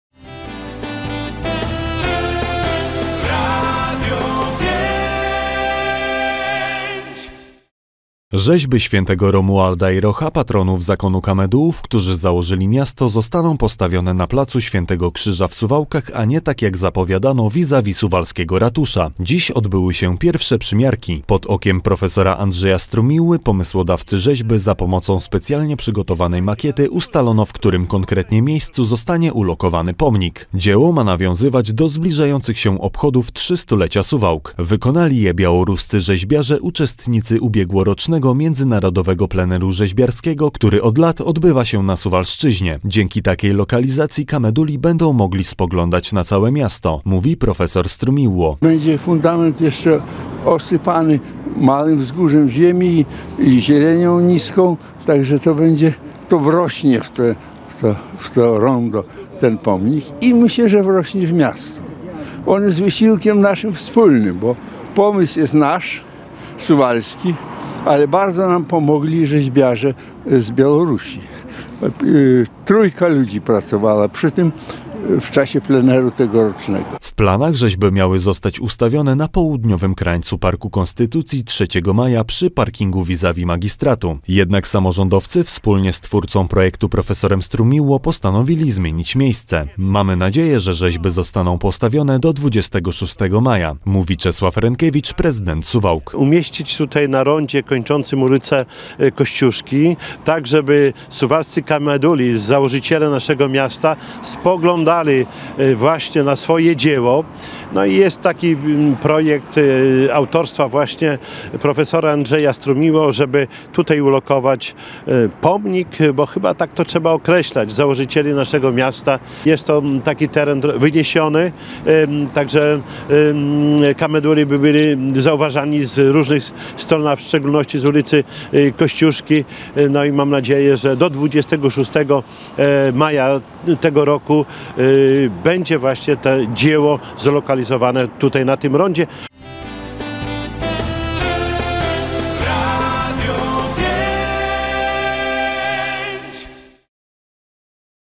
Kameduli będą spoglądać na miasto
Na placu  Świętego Krzyża w Suwałkach powstaną rzeźby świętego Romualda i Rocha, patroni zakonu kamedułów, którzy założyli miasto a nie tak jak zapowiadano vis-a-vis suwalskiego ratusza.
 Dziś(12.01) odbyły się pierwsze przymiarki. Pod okiem Andrzeja Strumiłły, pomysłodawcy rzeźby, za pomocą specjalnie przygotowanej makiety ustalono w którym konkretnie miejscu zostanie ulokowany pomnik. Dzieło ma nawiązywać do zbliżających się obchodów 300-lecia Suwałk. Wykonali je białoruscy rzeźbiarze, uczestnicy ubiegłorocznego, międzynarodowego pleneru rzeźbiarskiego, który od lat odbywa się na Suwalszczyźnie.
 - Dzięki takiej lokalizacji kameduli będą spoglądać na całe miasto - mówi profesor Strumiłło.

 W planach rzeźby miały zostać ustawione na południowym krańcu Parku Konstytucji 3 Maja, przy parkingu vis-a-vis magistratu. Jednak samorządowcy wspólnie z twórcą projektu profesorem Strumiłło  postanowili zmienić miejsce. - Mamy nadzieję, że rzeźby powstaną do 26 maja - mówi Czesław Renkiewicz, prezydent Suwałk.